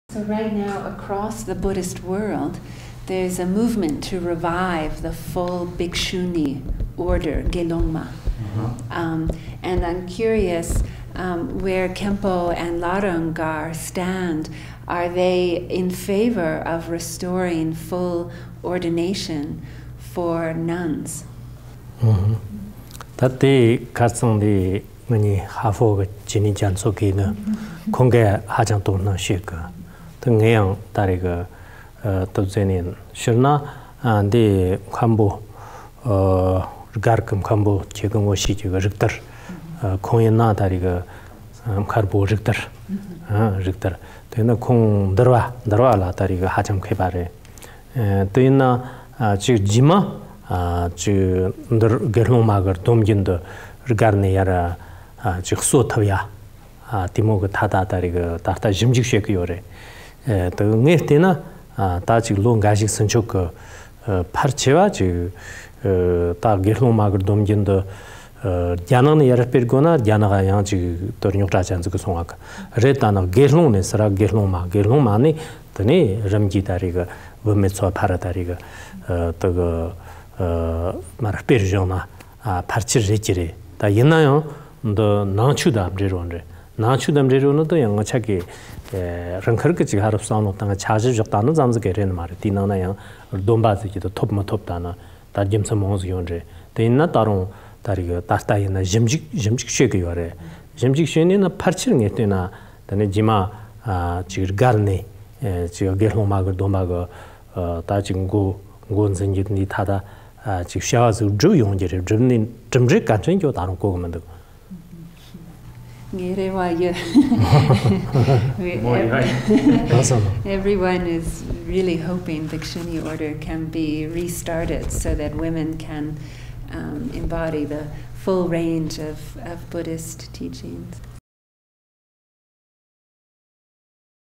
12 Restoring Bhikkhuni Ordination
2013 Buddhism in Today’s Scientific Age-A Conversation with Professor Gayley, Khenpo Sodargye